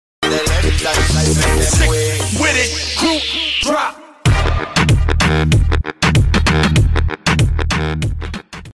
DROP | TIKTOK
Doing tiktok with my little one.. 😍😍😍